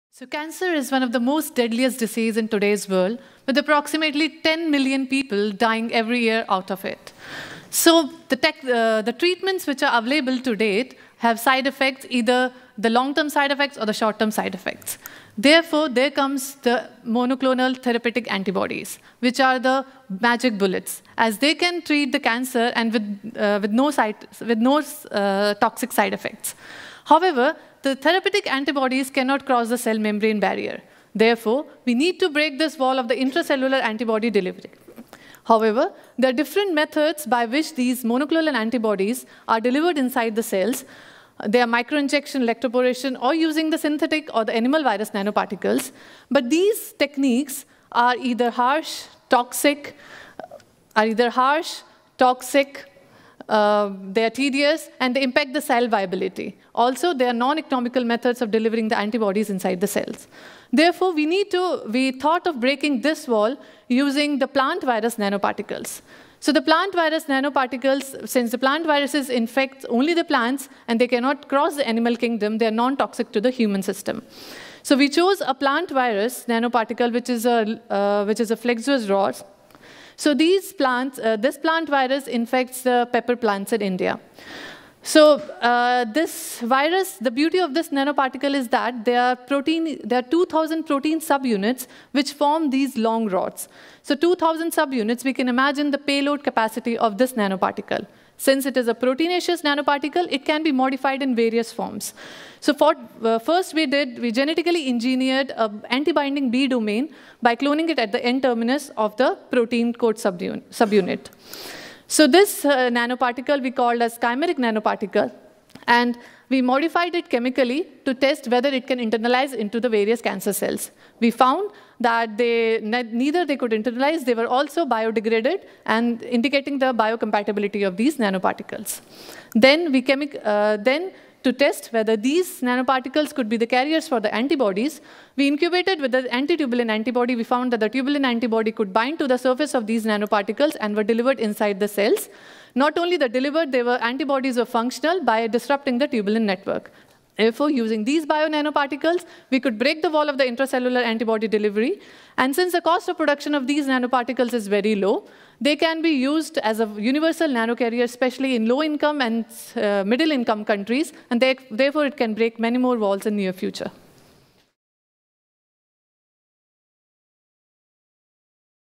Falling Walls Lab 2019 – Pallavi Sabharwal: Breaking the Wall of Intracellular Antibody Delivery
Pallavi Sabharwal, India